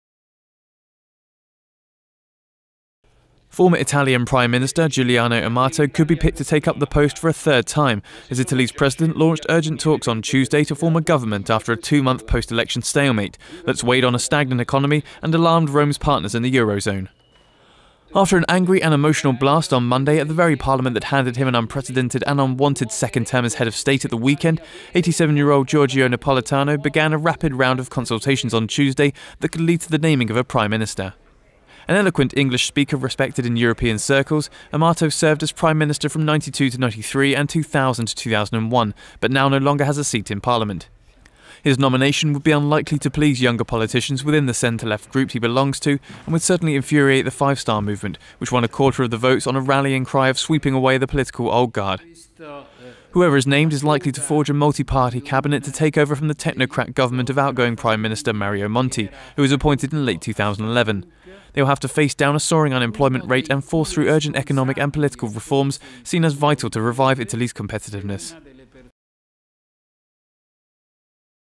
Amato Could Be Person Asked To Form Italy Stopgap Government
Former Italian prime minister Giuliano Amato could be picked to take up the post for a third time.